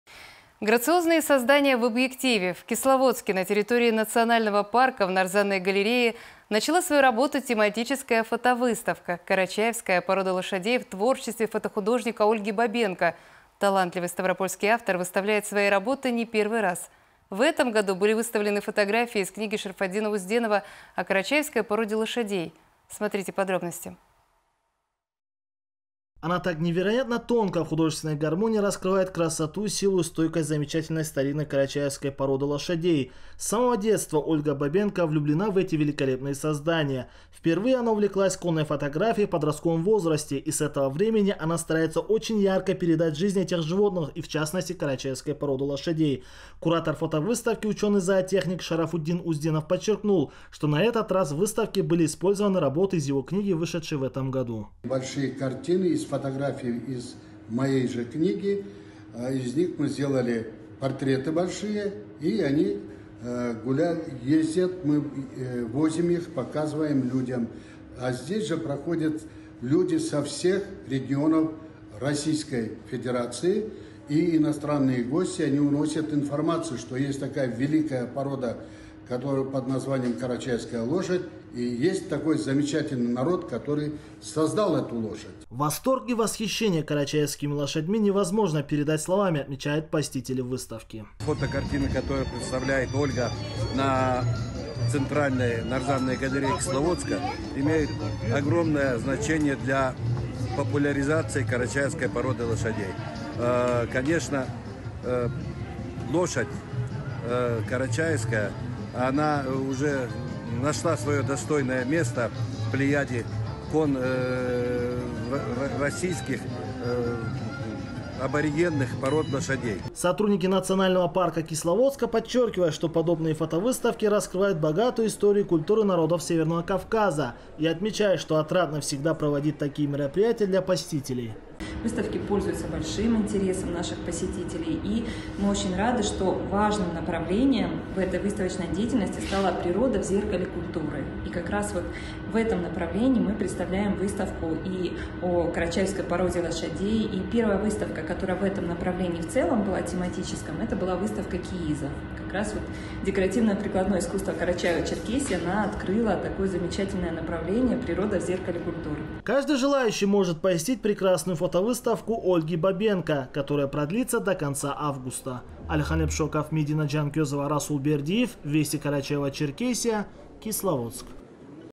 В Кисловодске открылась выставка "Карачаевская порода лошадей" фотохудожника Ольги Бабенко
Грациозное создание в объективе: в Кисловодске, на территории национального парка, в Нарзанной галерее, начала свою работу тематическая фотовыставка "Карачаевская порода лошадей" в творчестве фотохудожника Ольги Бабенко. Талантливый Ставропольский автор выставляет свои работы не первый раз. В этом году были выставлены фотографии из книги Шарафутдинова Узденова о карачаевской породе лошадей